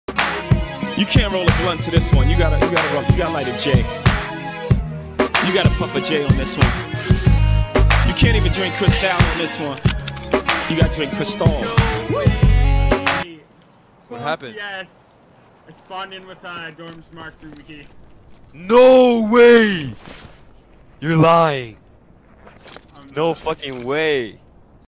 POV: you spawn with dorms marked room key on christmas
This is the most sought-after key at the start of wipe and I got lucky and spawned with it on my scav.